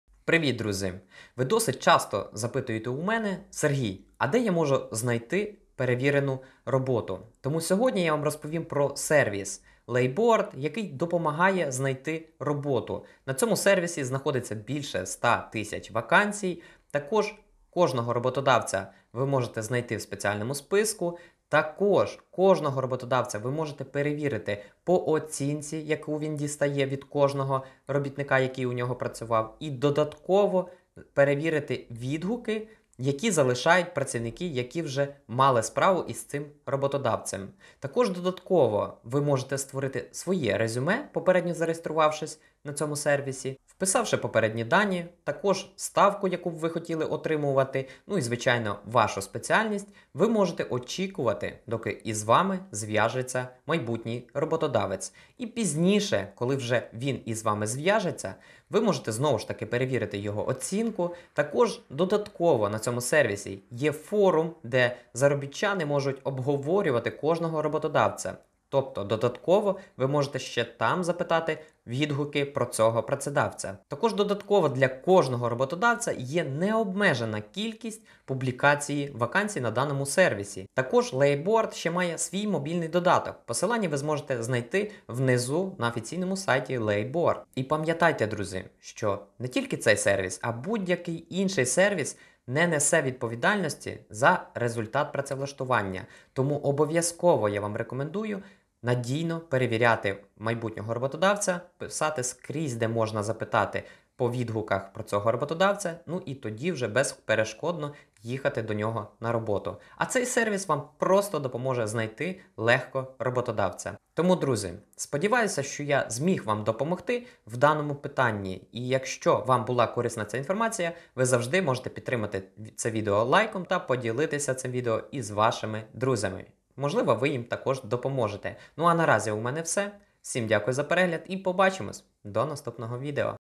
Обзор сайта по работе за границей - Layboard.com
Друзья, сегодня я осмотрел сайт по трудоустройстве за рубежом - layboard.com

Из интересного:
- Больше 100 000 вакансий за границей
- Есть база резюме (можно зарегистрировать резюме и ждать пока Вам перезвонят)